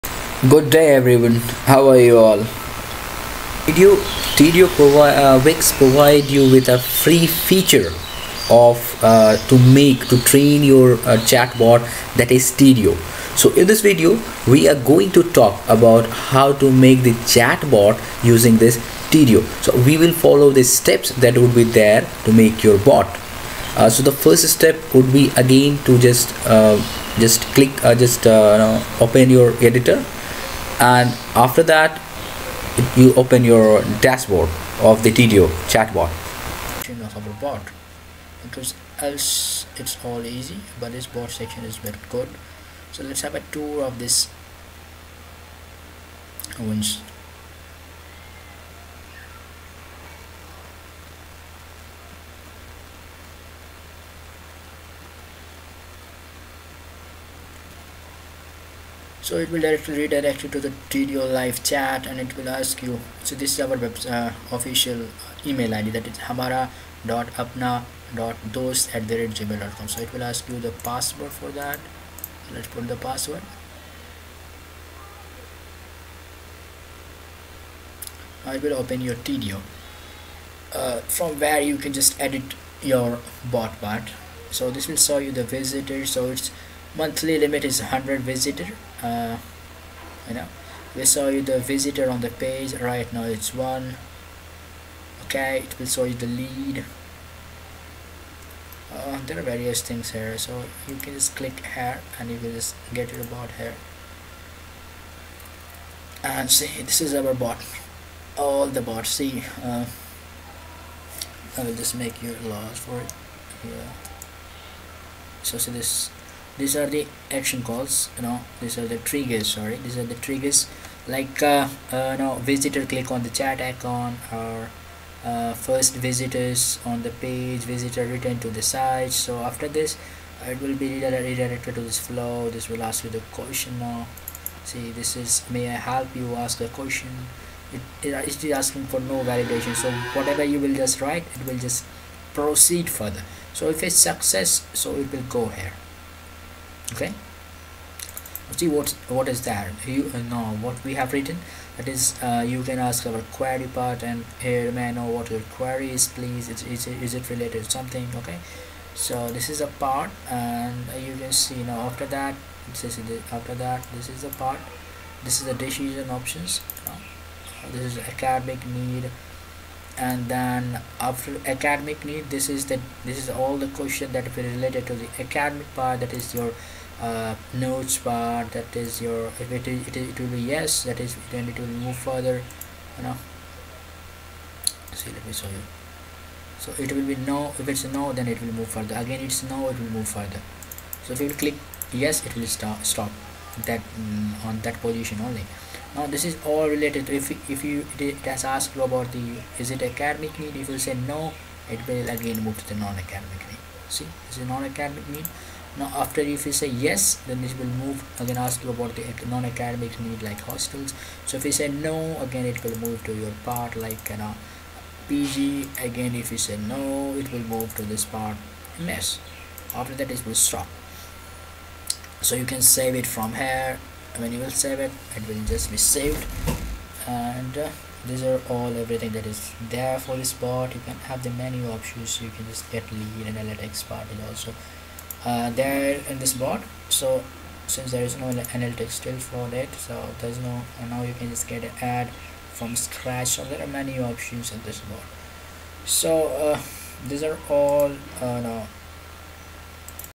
How to integrate a Wix website with the Tidio both?
In this video, you are going to know how to design a Tidio Chatbot bot for your Wix website.